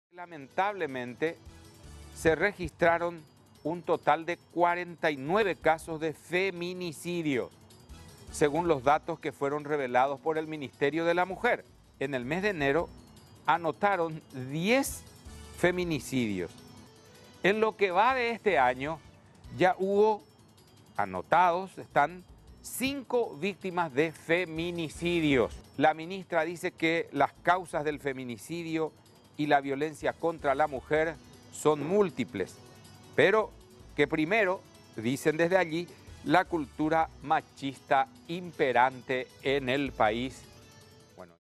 Feminicidios aumentaron en Paraguay - ÚH
Un total de 49 casos de feminicidio son los registrados por el Ministerio de la Mujer en el año 2017 y estos números lo convierten en el año más violento para las mujeres.

La ministra Vallardi sostuvo que el mes con más casos de asesinatos es el mes de enero, ya que el año pasado 10 feminicidios fueron registrados:
en el año 2016 hubo 39 casos en total; en lo que va de este año ya suman 5 los casos de mujeres asesinadas; además, la ministra declara que varios son los factores que impulsan a llegar a este acto, pero que el factor determinante es el machismo imperante en nuestra sociedad.